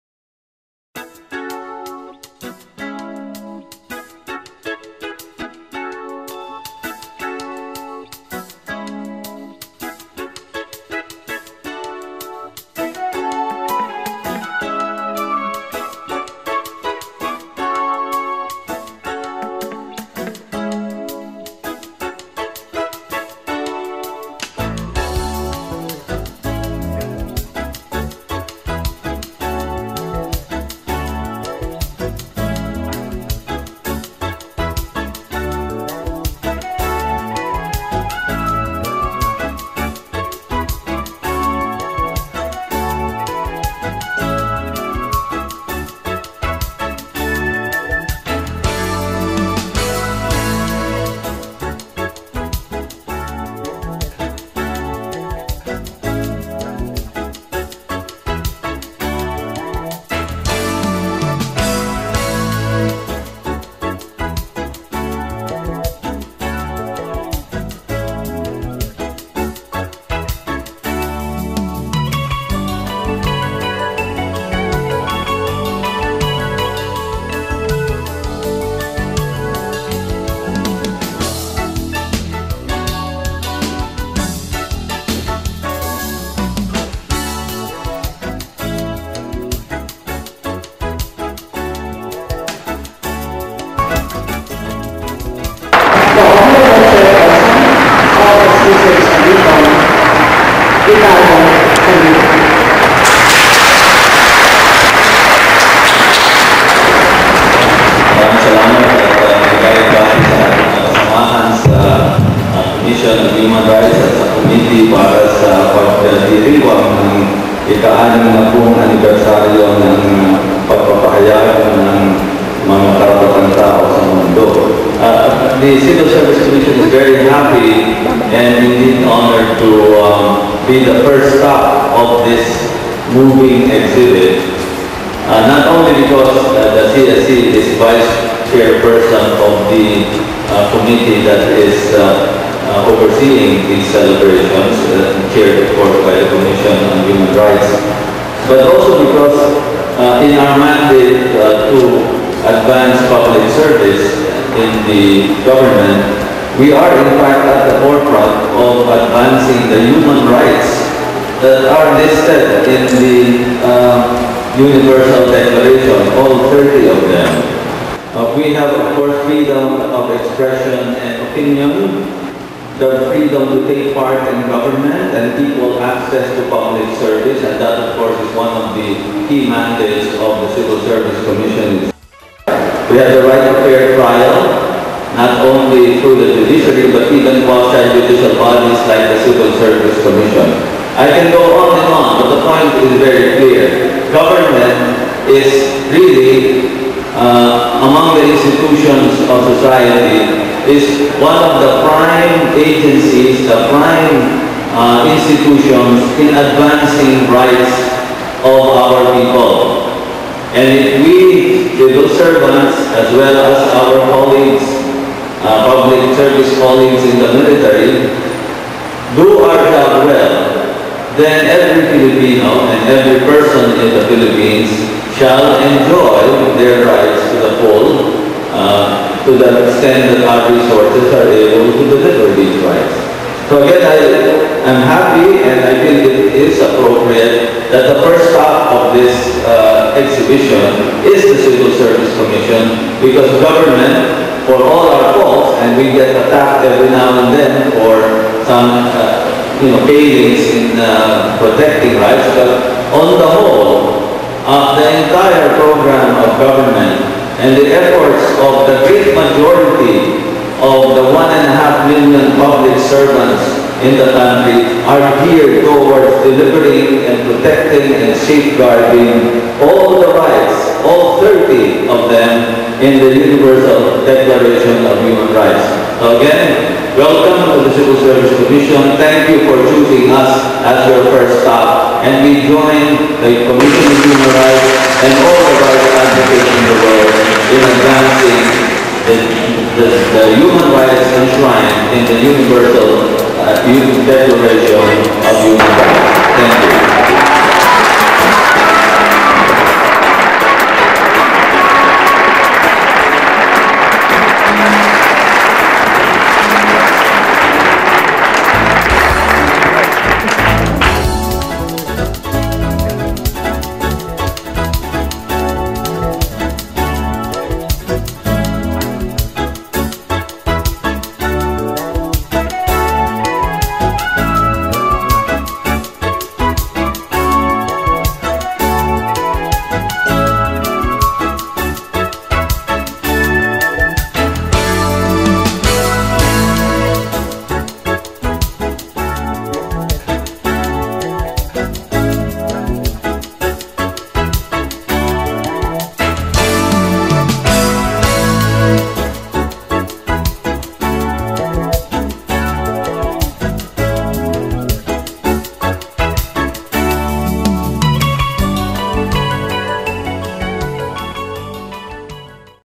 RLS
Speech delivered by CSC Chairman Ricardo L. Saludo during he 60th Anniversary Celebration of the Universal Declaration on Human Rights.